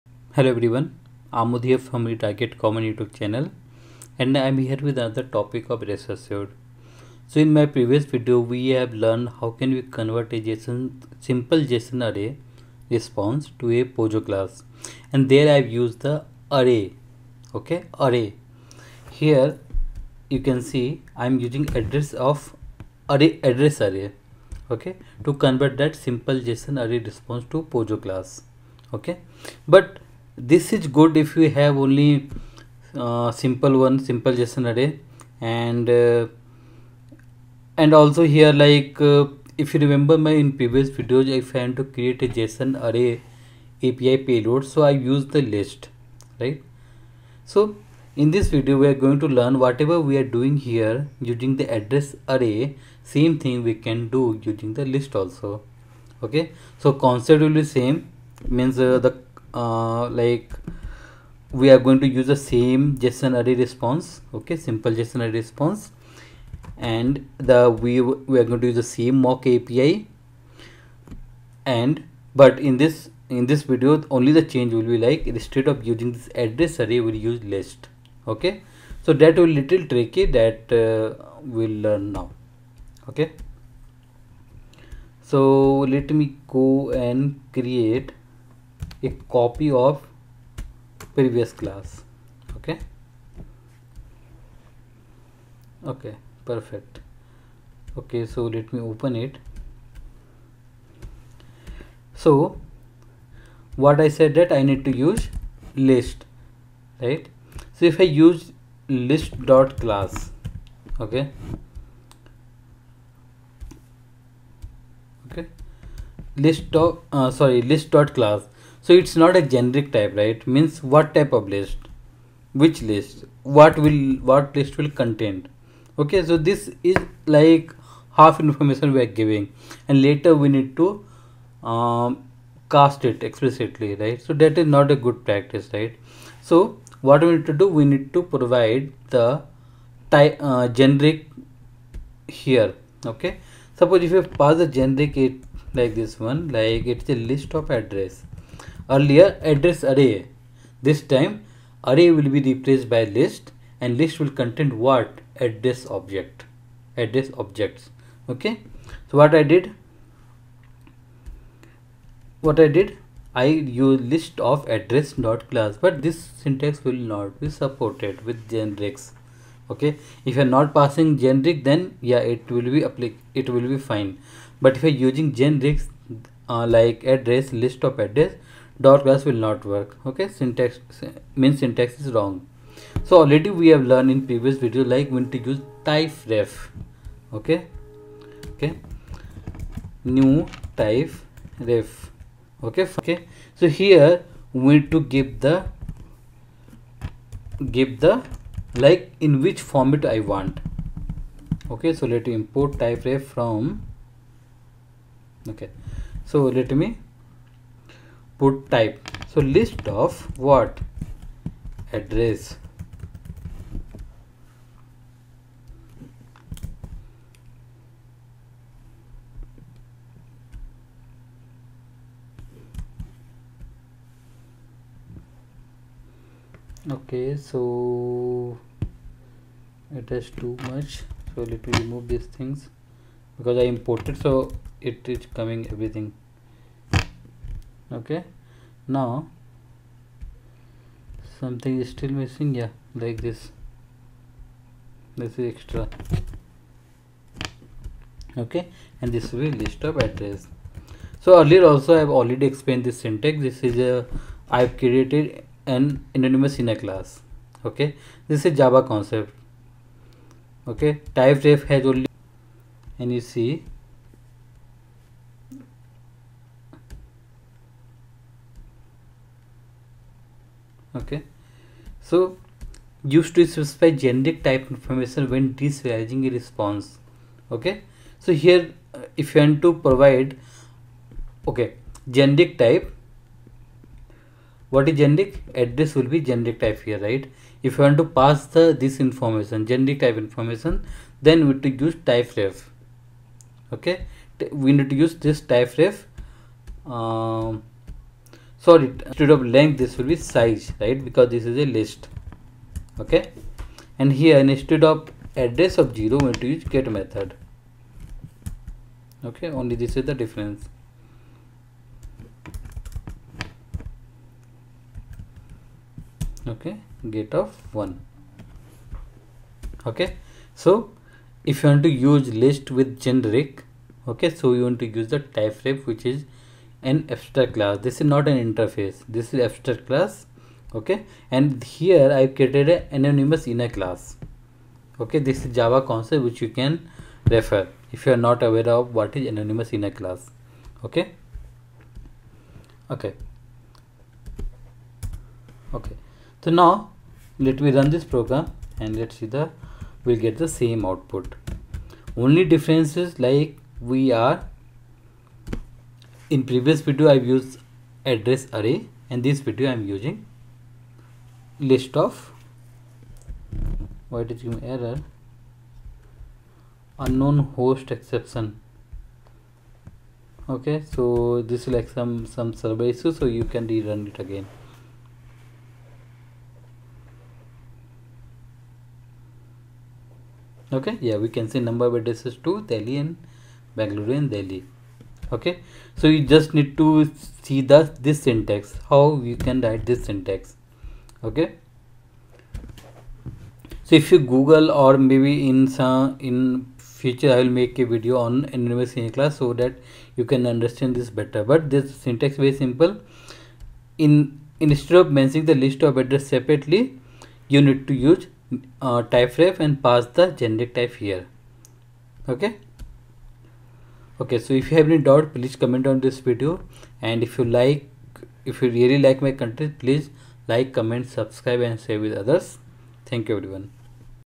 57. Convert Simple JSON Array Response To POJO Using Java List To Extract Value From Response
There are several ways of extracting values from JSON responses. We have already learned to extract values from JSON responses using JSON Path. We can do the same using POJO classes as well.

We learned to convert a simple and nested JSON object to POJO to extract values in the previous videos.

Now, let's learn to convert a simple JSON array response to the POJO class using the Java List concept to extract values.

If you do not like my content please share your feedback so that I can improve myself.

Thanks
Amod